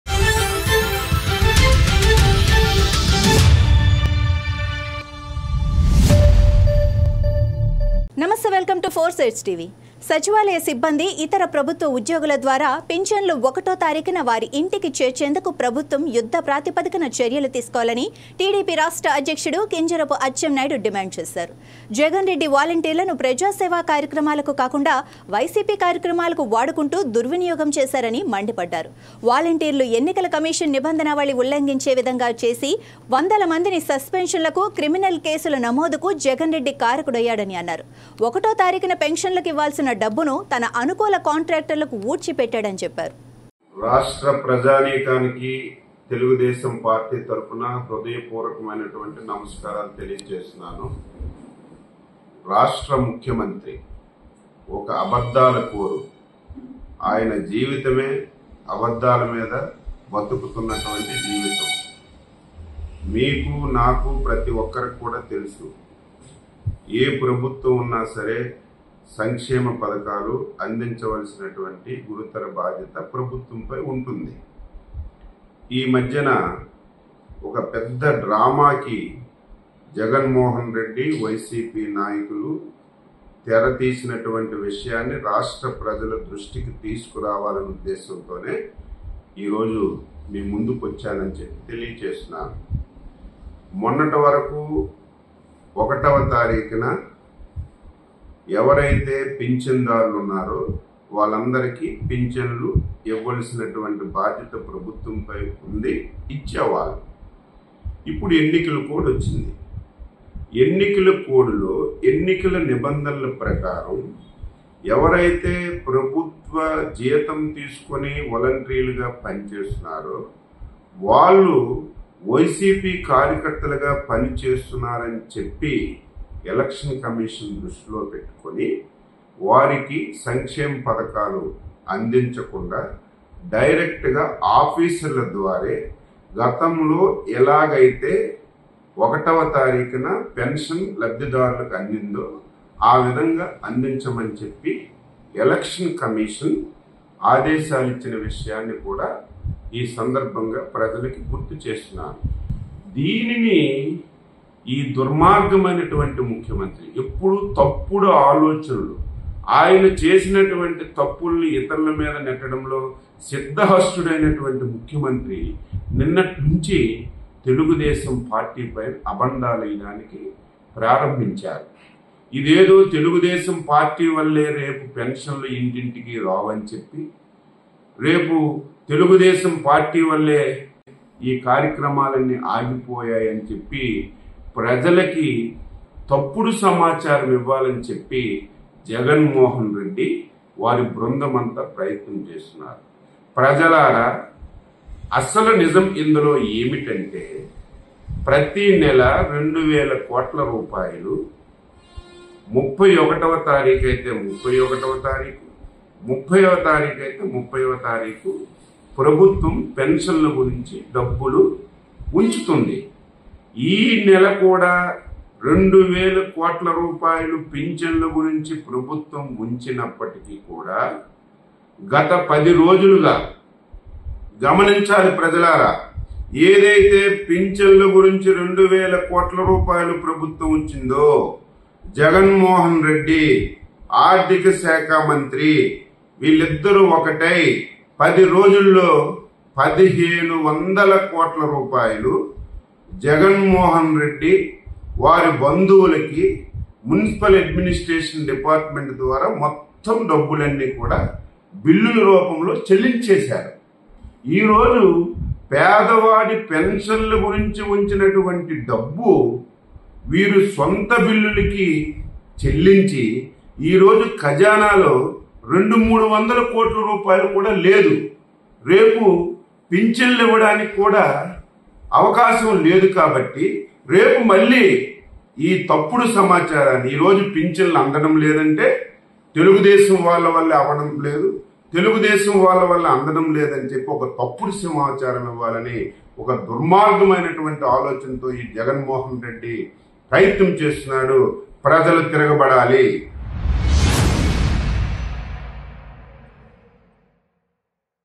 టీడీపీ అధికారంలోకి రాగానే ఇంటివద్దకే పింఛన్‌ || TDP State President Atchannaidu
TDP state president Kinjarapu Atchennaidu demanded that the government should take warlike measures to bring Finchans to their homes by the secretariat staff and other government employees. Jagan Reddy alleged that the volunteers were misused by using them for YCP activities instead of public service activities. He said Jagan Reddy was responsible for the suspension of hundreds of volunteers and the registration of criminal cases by violating the rules of the Election Commission. On one date, he said that he had siphoned off the money due for pensions to his favored contractors.